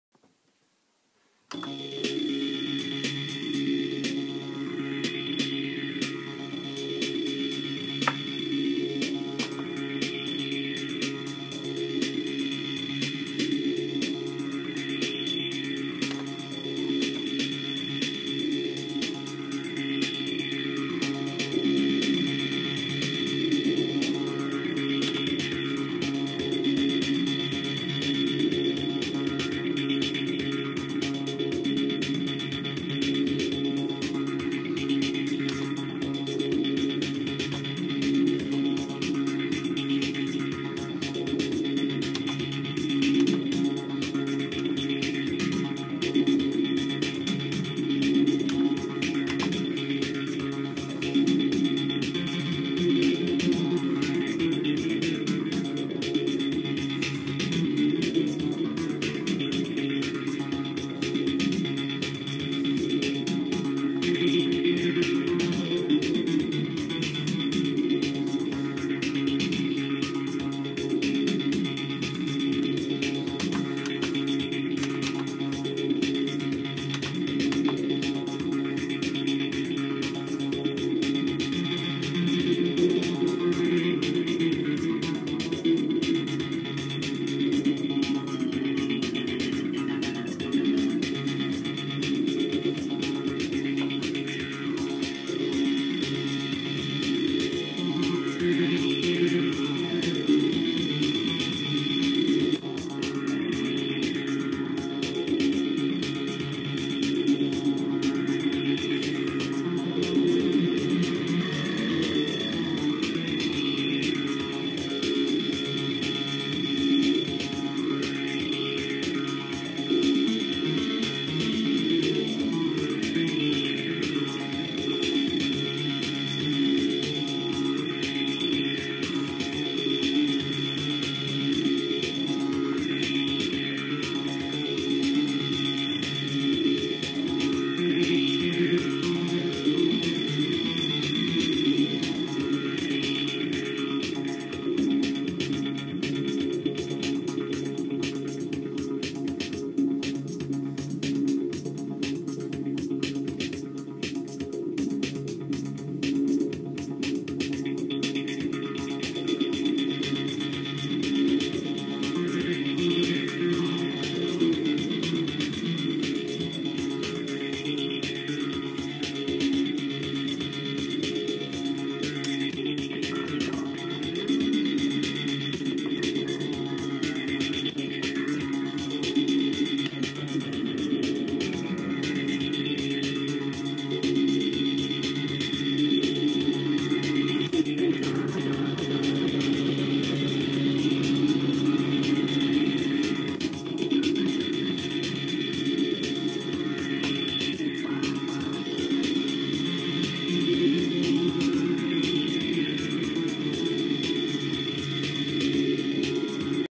Zaquencer + Shruthi + Alesis SR-16
I've been using the zaq for less than an hour and I don't have the overlay yet so i'm pretty clueless about using it but it's a fun little sequencer to mess about on. 100% computer-less music!